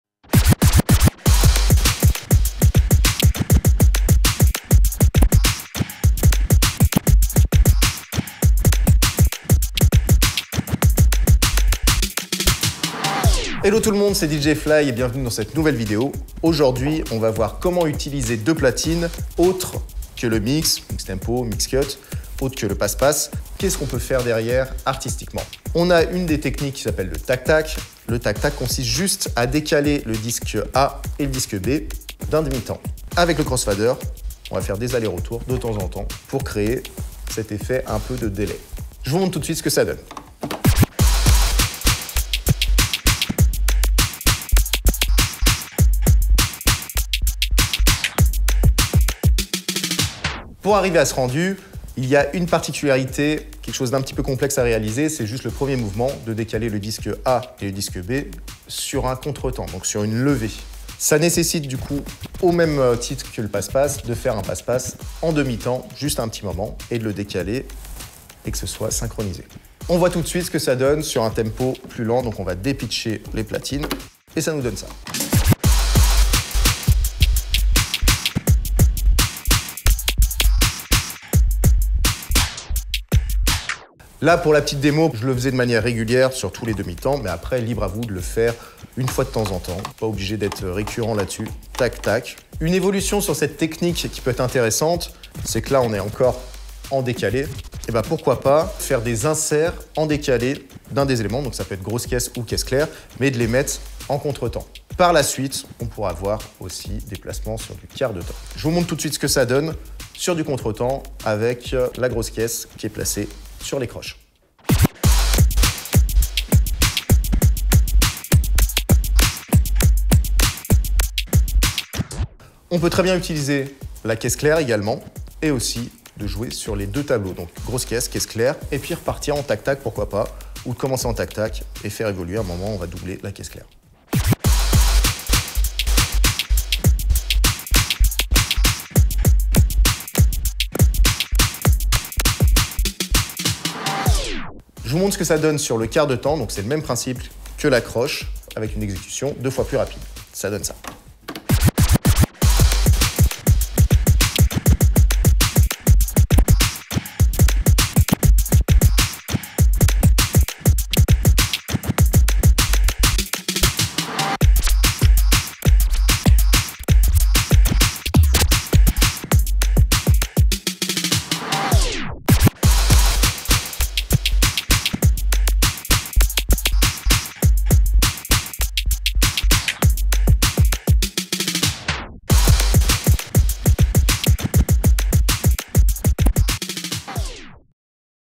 RANE & DJ FLY : Fondamentales du Scratch ! #8 Le TAC-TAC ! (Boite Noire du Musicien)
Dans ce 8ème épisode de tutoriels sur le SCRATCH, apprenez le TAC-TAC !!

DJ Fly, est DJ français de hip-hop et electro. Il joue sur une régie RANE composée de 2 platines TWELVE et une table de mixage Seventy Two mk2. En 2008 et en 2013, il remporte le championnat du monde DMC ! Il nous fait le plaisir de proposer cette série d'initiation et de perfectionnement au scratch.